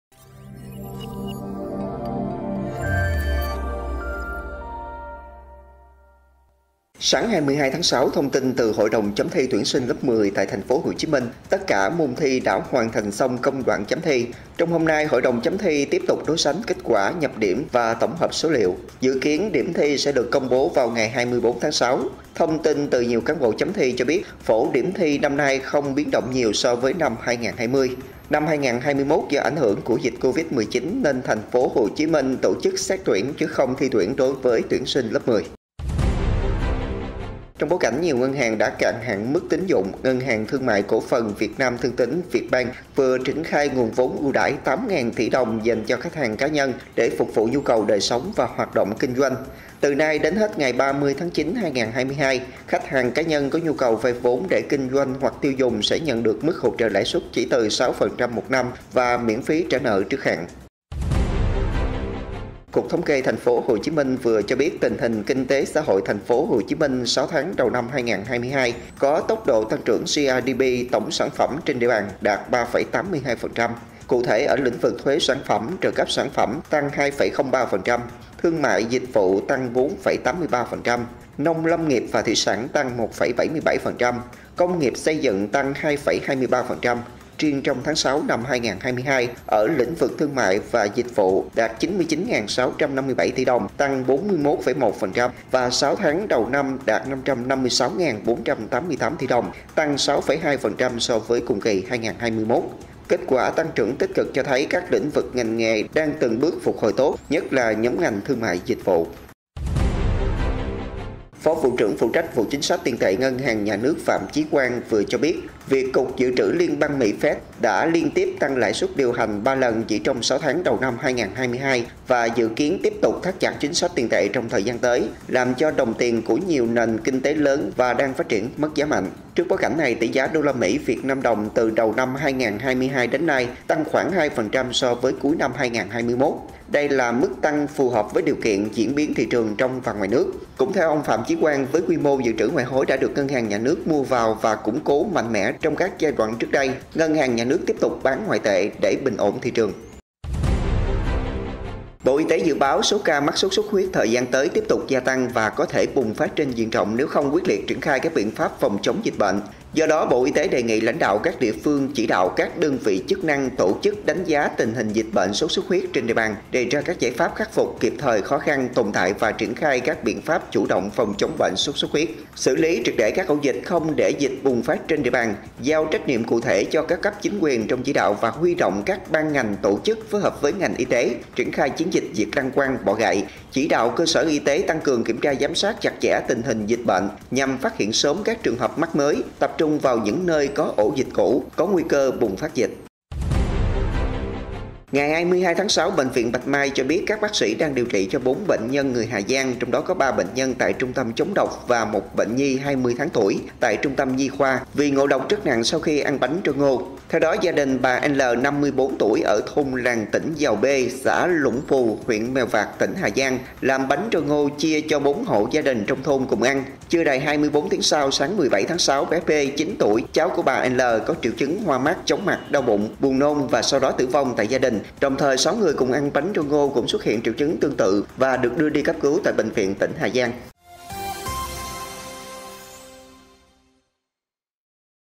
Điểm tin SGGP Online ngày 22-6-2022
Nửa năm 2022, tăng trưởng kinh tế TPHCM đạt 3,82%; Dịch sốt xuất huyết vào mùa cao điểm; Ăn bánh trôi ngô, 1 trẻ nhỏ tử vong và 4 người nguy kịch… là những nội dung đáng chú ý có trong Điểm tin SGGP Online ngày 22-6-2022.